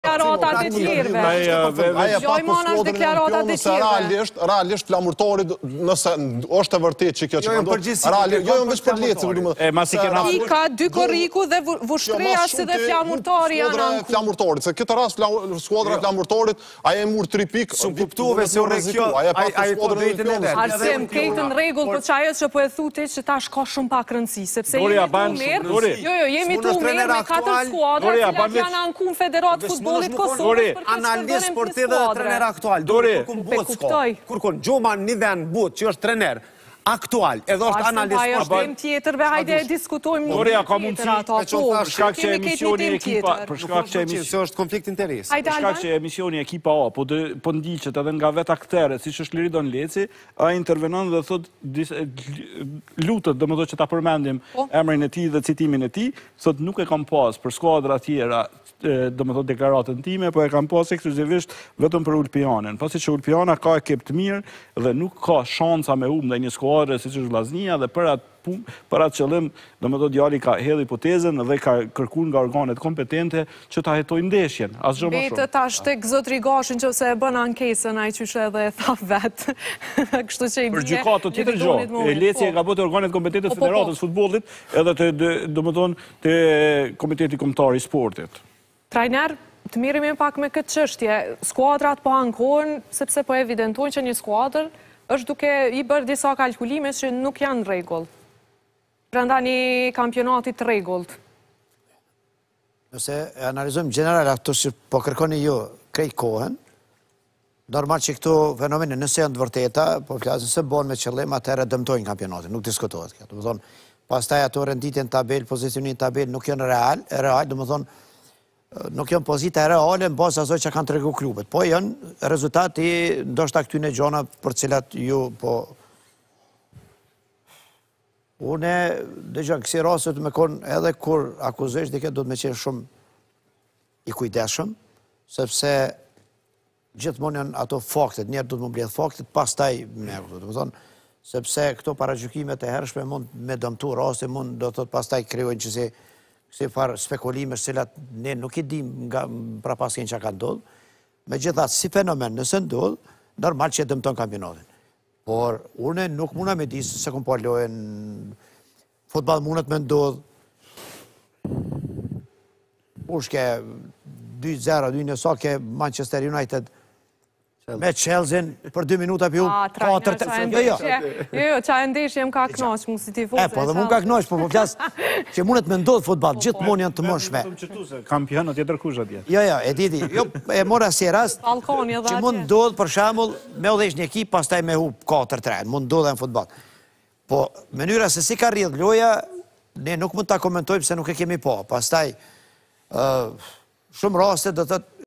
Leci inkuadrohet në Ekipi A:Ulpiana ka ekip të mirë, nuk mund të humbin ndaj një skuadre si Vllaznia
Follow ATV on Facebook:

 / atvks

Follow ATV on Instagram:

 / atvlive.tv